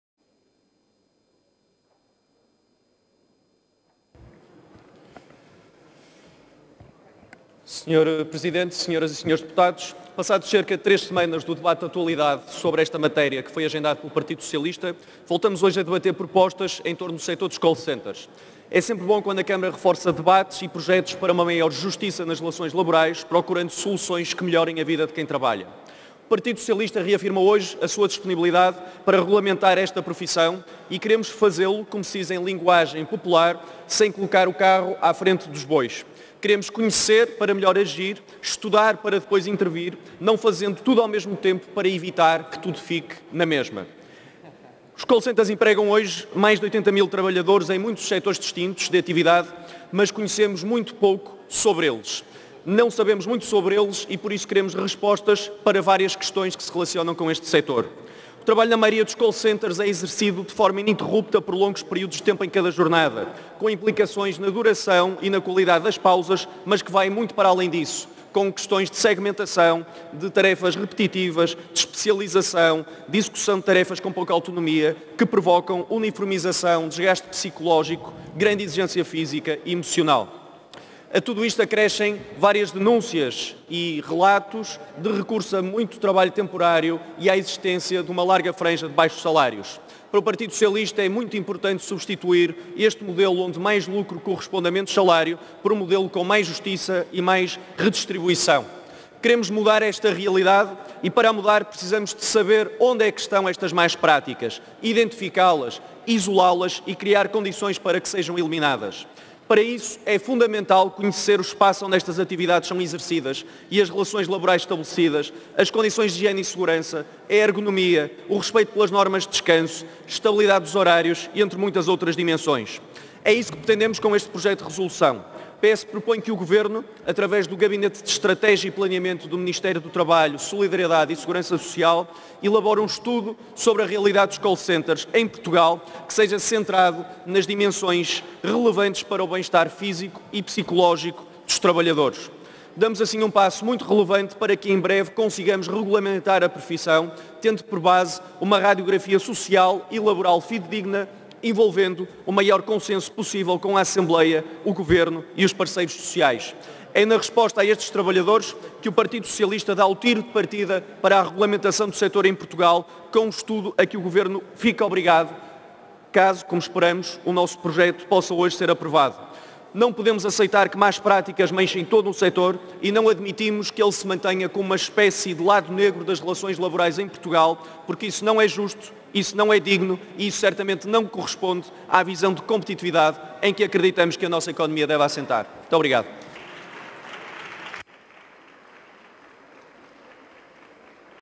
15-03-2019 - Debate Parlamentar | Regulamentação do trabalho em Call Center | Tiago Barbosa Ribeiro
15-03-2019 | Intervenção do Deputado Tiago Barbosa Ribeiro, no âmbito dos Projetos de Resolução (PS, PCP, PEV e BE) Regulamentação do trabalho em Call Center